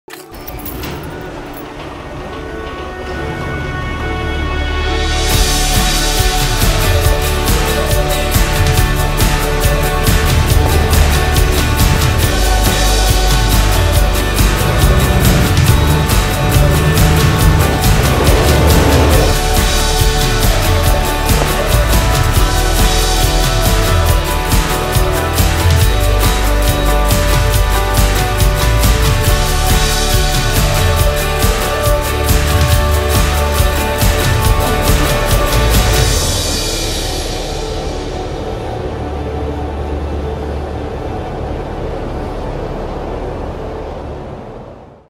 Overlanding Journey with Trayvax
Trayvax, USA manufacturer of minimalist wallets and gear, journeys through overlanding to explore and get away.